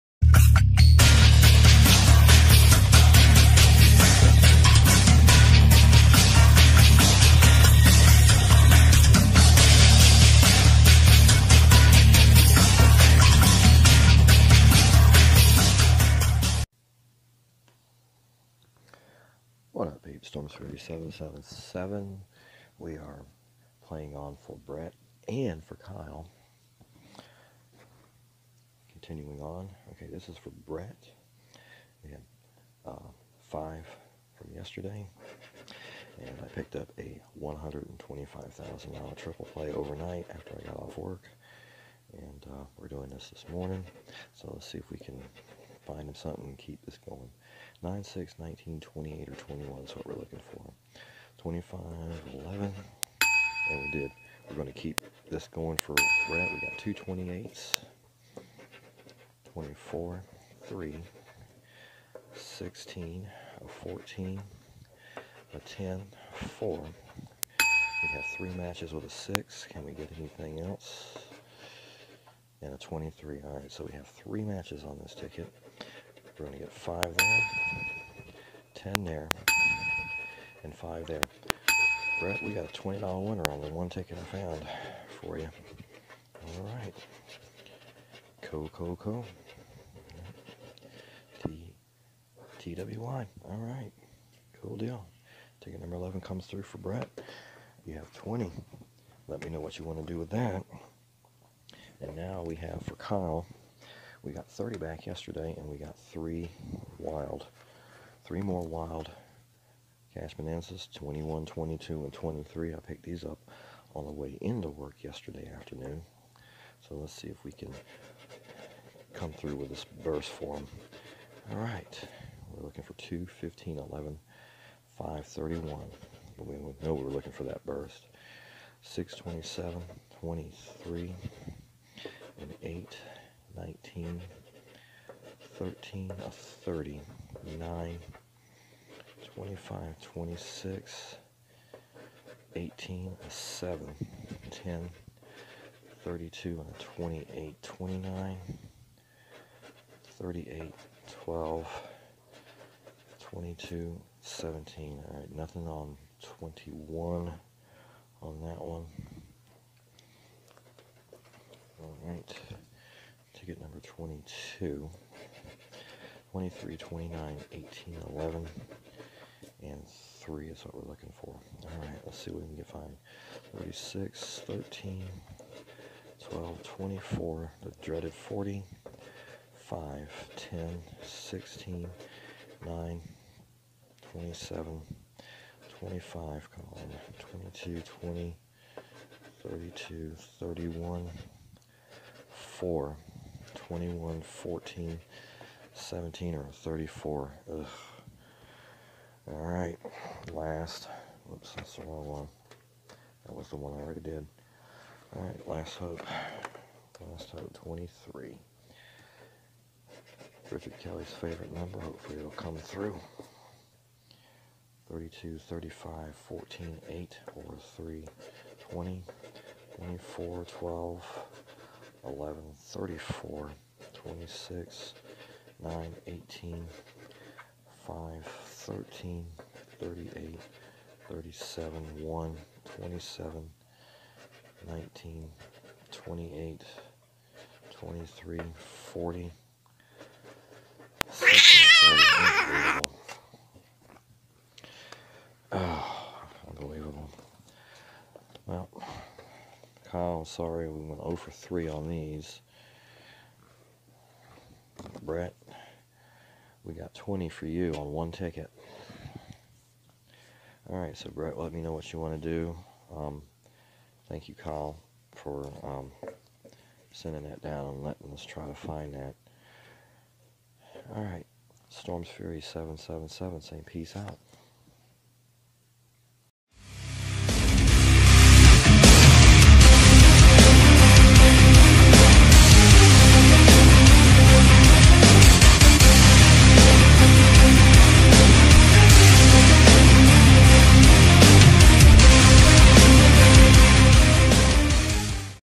Playing on For Brett and Kyle! Triple Play and More Wild Cash Bonanza!
Brett elected to play on with a 5 dollar ticket and Kyle elected to continue the burst hunt with 3 more Wild Cash Bonanzas...let's keep this going!